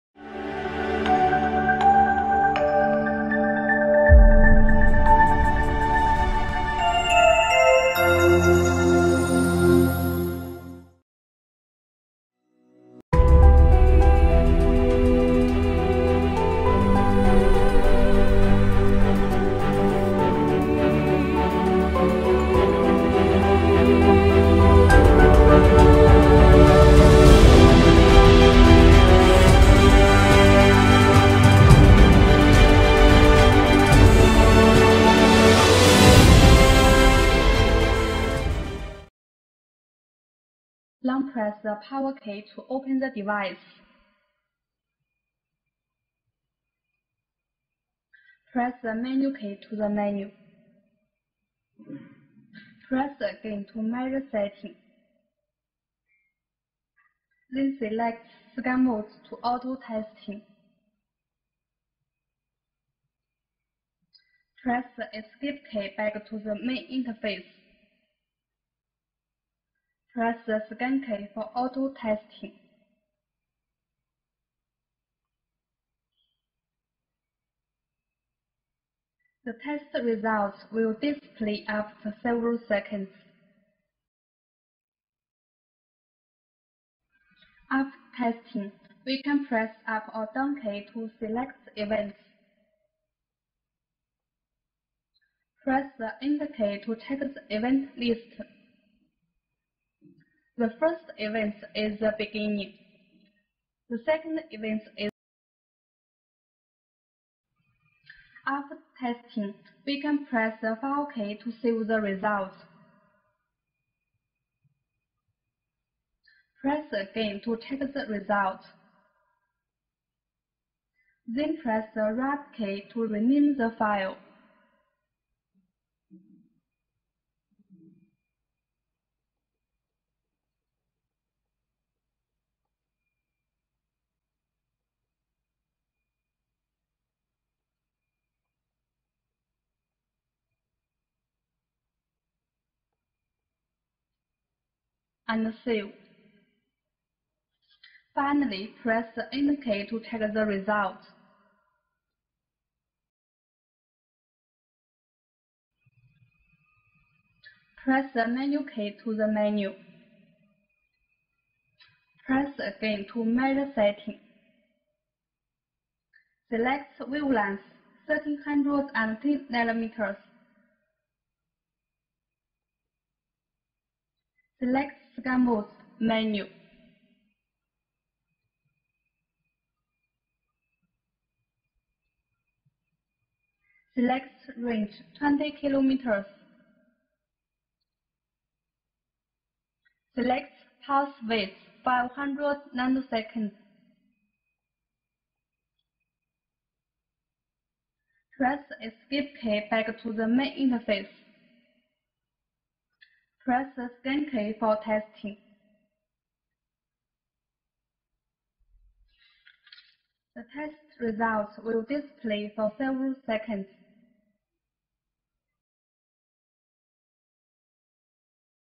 AOR 500 OTDR | Stanlay
AOR500 OTDR is palm rugged, handheld, and easy to operate OTDR provided with a 3.5-inch TFT color screen mainly for use in FTTx projects or CATV networks to help technicians measure the fiber length and attenuation loss.

Multi-wavelength capability: AOR500 OTDR is a modular OTDR which can include 2 to 3 test wavelengths based on user selection. Module options include options for Multimode optical fiber, Single-mode optical fiber and PON networks.